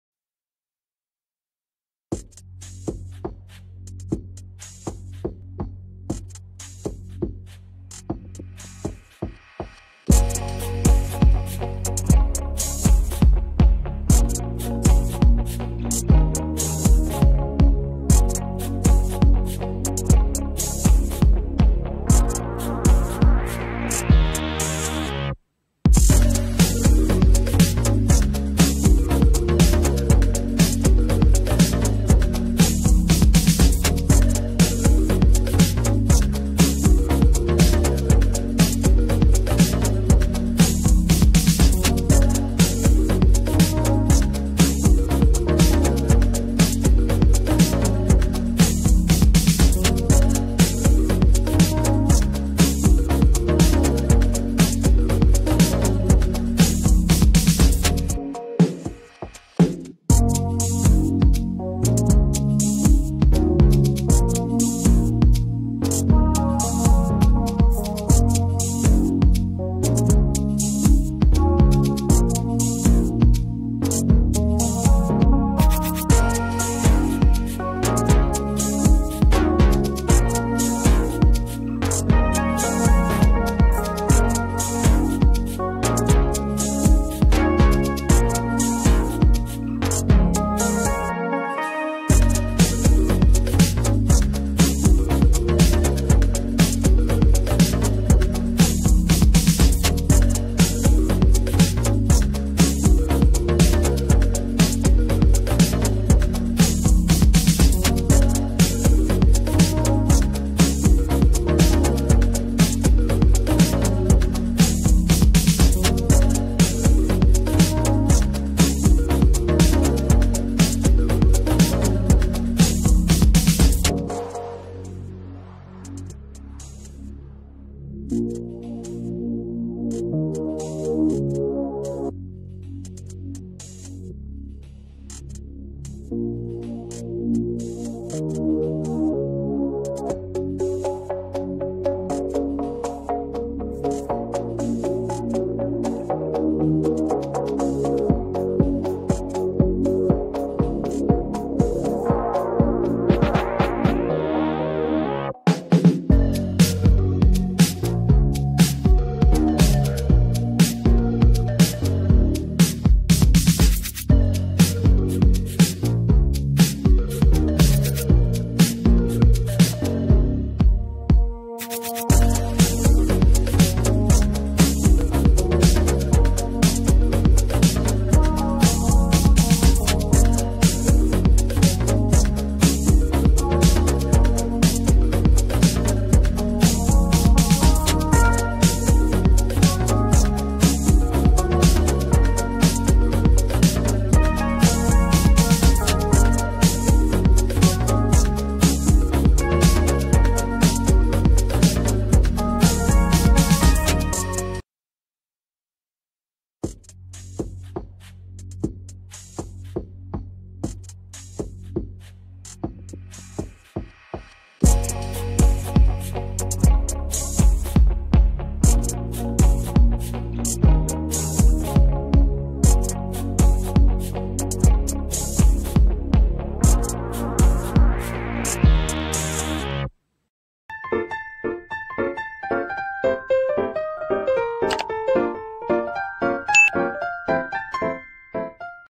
wallmate/wall hanging craft ideas/paper wallmate/paper craft
Paper flower wall hanging is shown in this video

kagojer wallmate new
paper crafts wall hangings
paper wallmate
wall hanging craft ideas
কাগজের ফুল